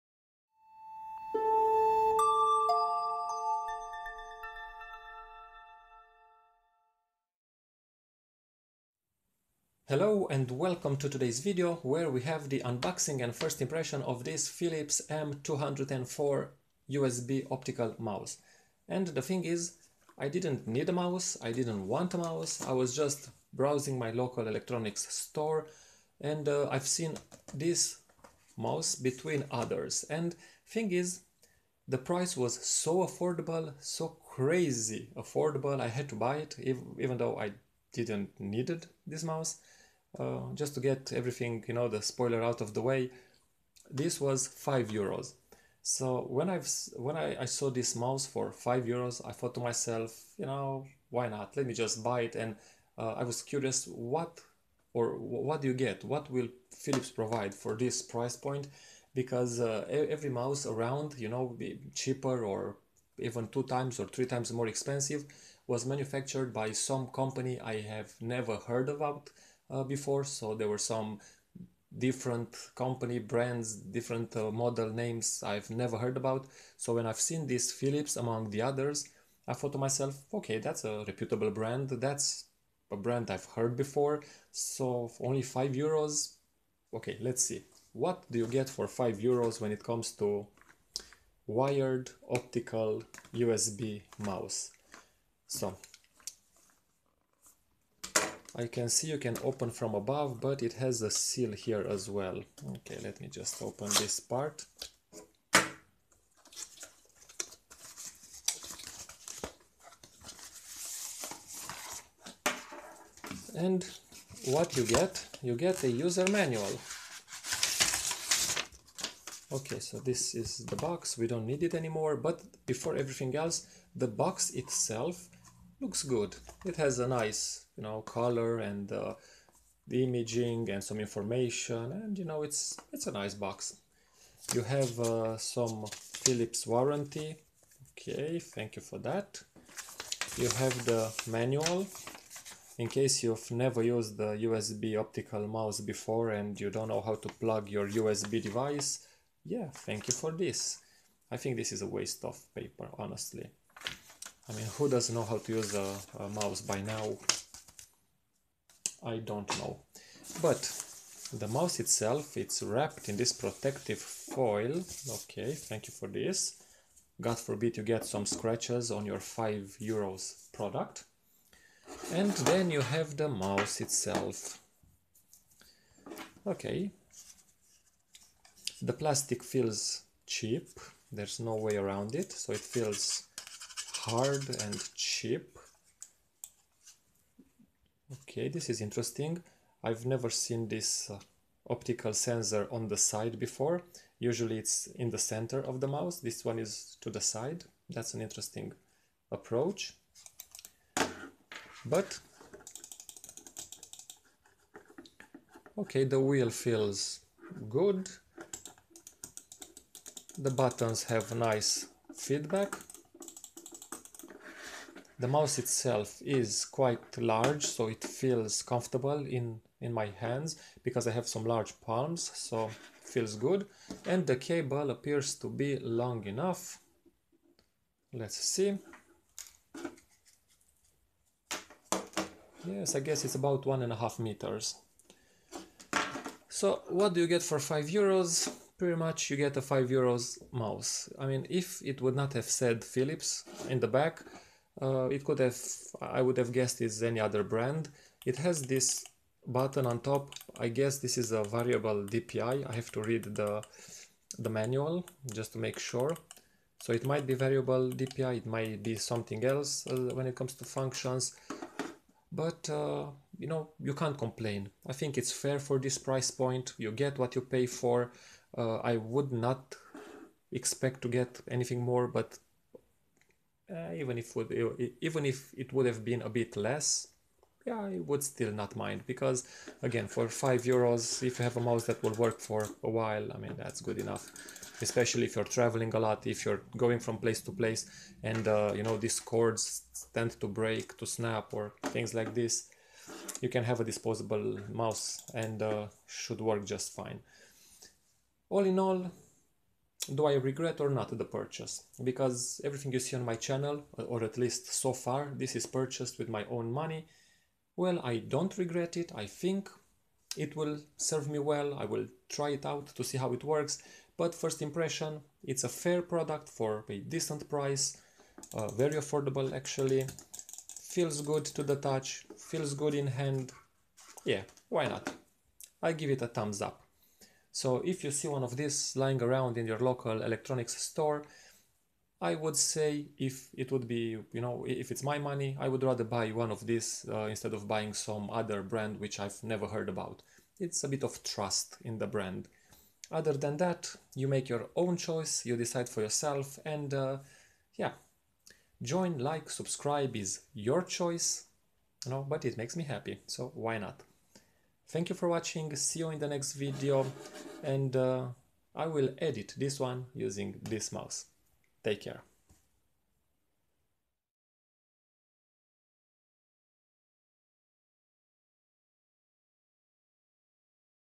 PHILIPS made this crazy affordable mouse - here is what I think of it ... (M204 / SPK7204)
Much appreciated!

This video presents the unboxing and first impressions of an affordable wired optical mouse by PHILIPS. I bought this mouse with my own money so I have no incentive to say anything but my own opinion. Spoiler: it seems like a good mouse, especially considering its purchase price. The rest of my impressions you'll find in the video. Enjoy !

Brand: PHILIPS
Model: M204 / SPK7204
Price: around 5-10 $